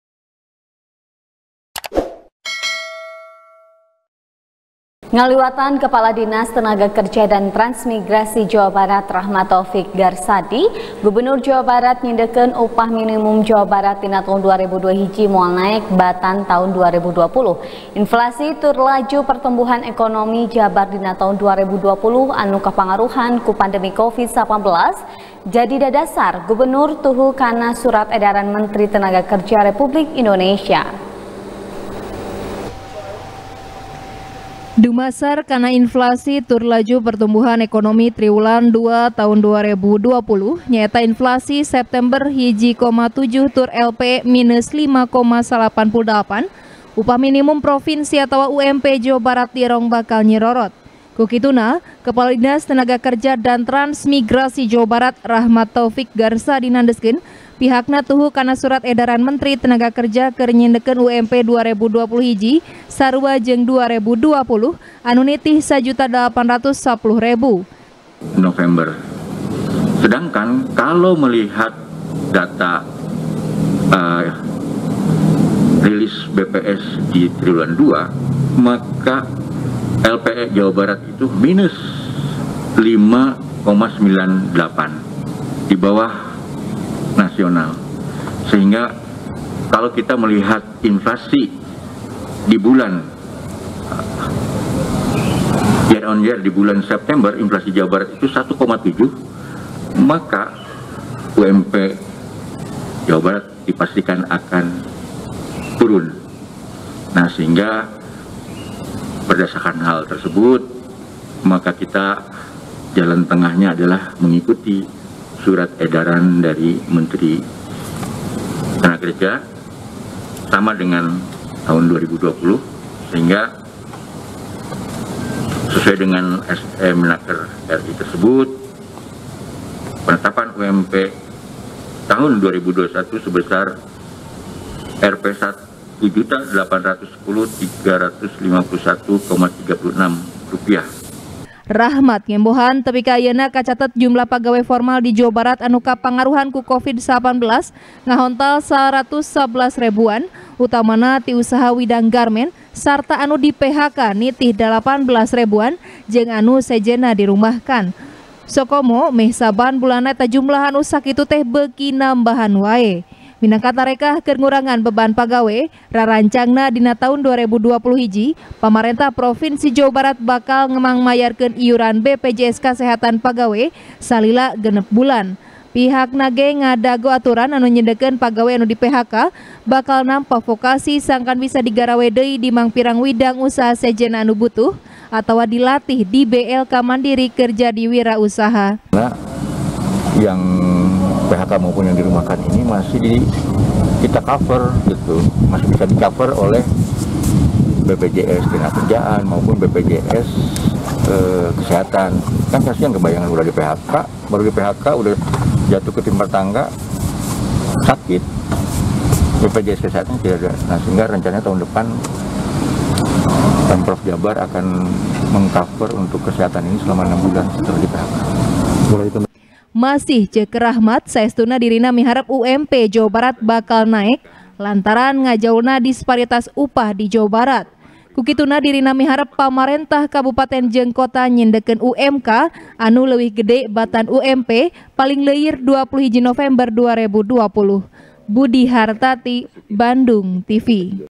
Luyu Kana Edaran Menteri, Ump Jabar Teu Naek | TP | BANDUNGTV
TP | BANDUNGTV  | ASTRI CAHYANI | Ngaliwatan Kepala Dinas Tenaga Kerja Dan Transmigrasi Jawa Barat, Rachmat Taufik Garsadi, Gubernur Jawa Barat Nyindekeun, Upah Minimum Jawa Barat Dina Taun 2021 Moal Naek Batan Taun 2020. Inflasi Tur Laju Pertumbuhan Ekonomi Jabar Dina Taun 2020 Anu Kapangaruhan Ku Pandemi Covid-19, Jadi Dadasar Gubernur Tuhu Kan Surat Edaran Menteri Tenaga Kerja Republik Indonesia.